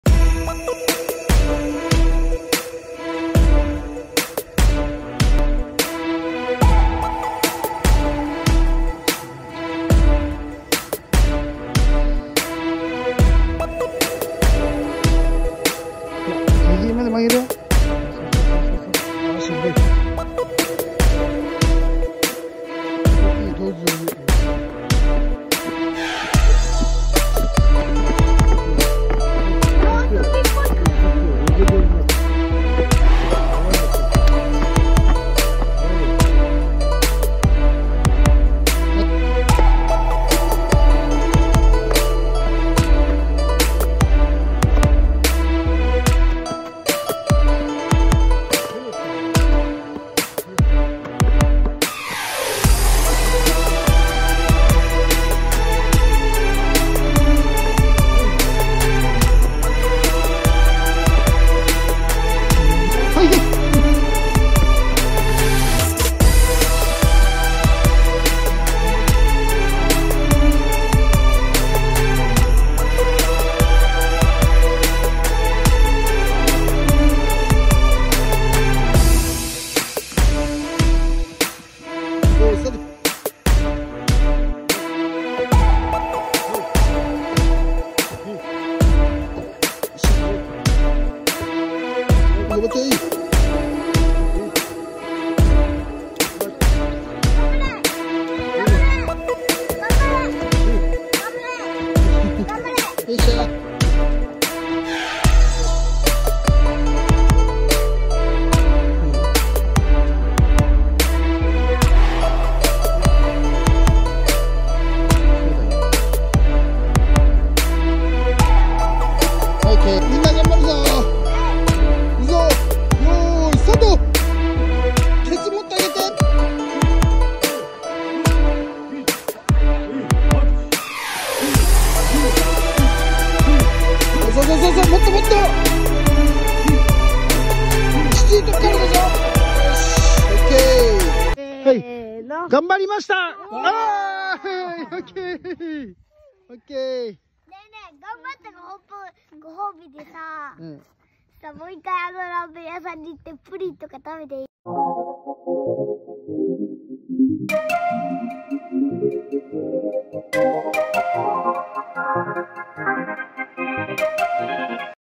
上田道場合宿 魂の朝稽古🥋Ueda Dojo Camp👊
上田道場2日間の合宿、朝稽古の様子です。
運動能力を上げる為に大切な体力作りの動画です！！
稽古は朝7時から８時３０まで１時間半たっぷり！
稽古後の皆んなでお風呂や、ご飯は最高でした😁👍
5歳児達は親元をはじめて離れ頑張りました！